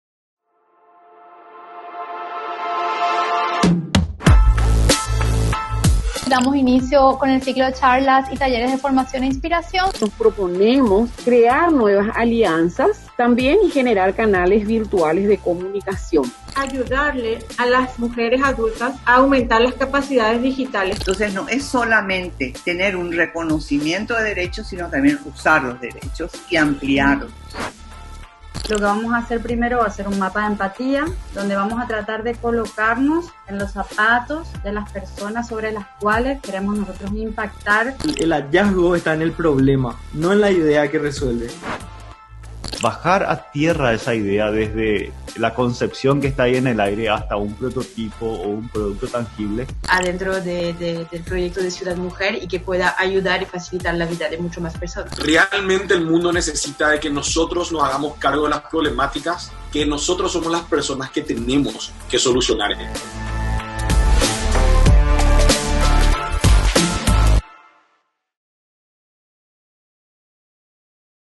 Hackathon - Kuña Mbaretech
Así vivimos los 3 días de hackathon de KuñaMbaretech 🎉

Gracias a las y los participantes, mentores, expositores y todo el equipo que hizo posible este concurso 100% virtual.

Sigamos construyendo soluciones para los desafíos que viven las mujeres cotidianamente 🙌✨